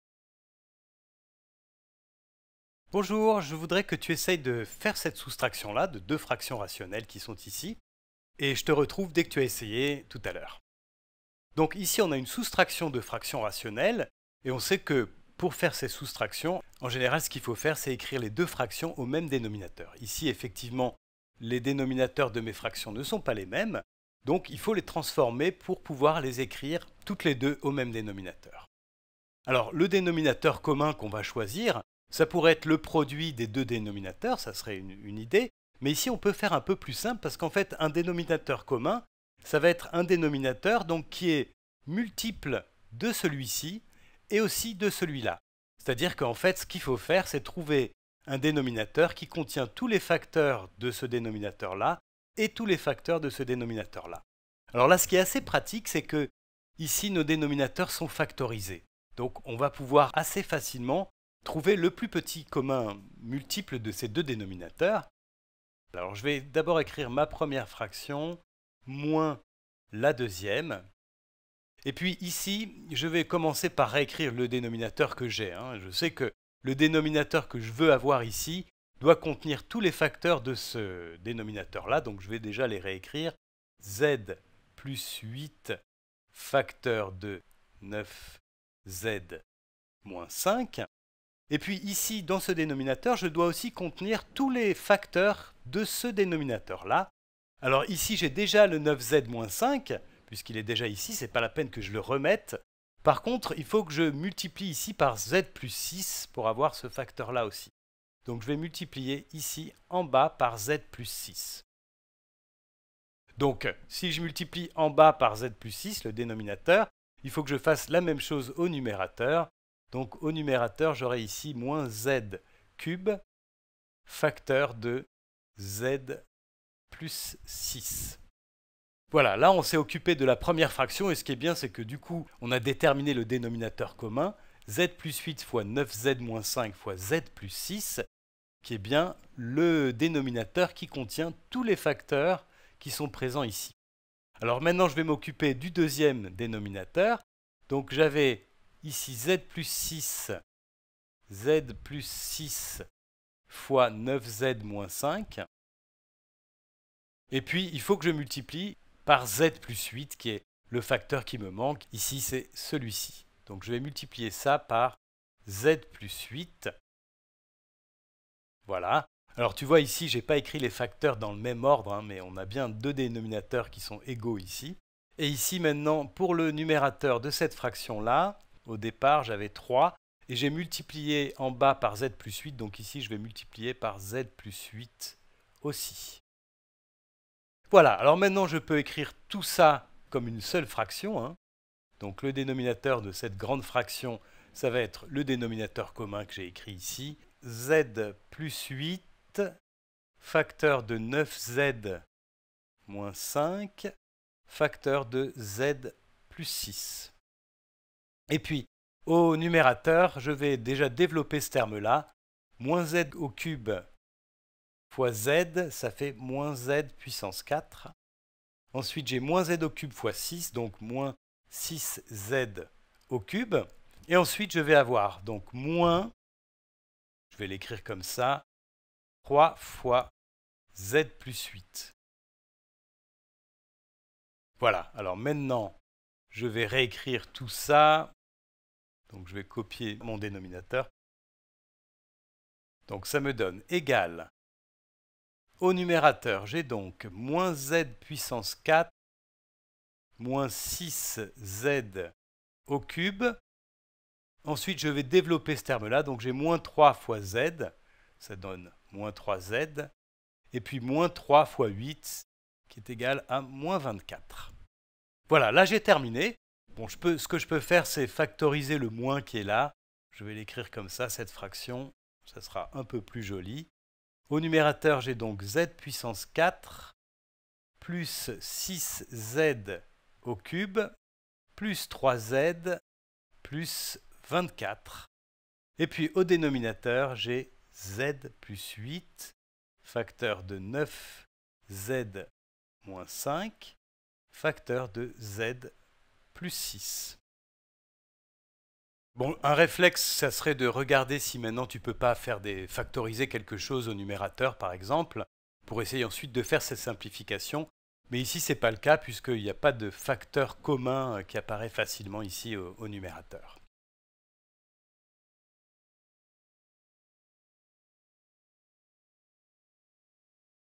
Soustraction de fractions rationnelles dénominateurs factorisés
Comment soustraire deux fractions rationnelles dont les dénominateurs sont factorisés.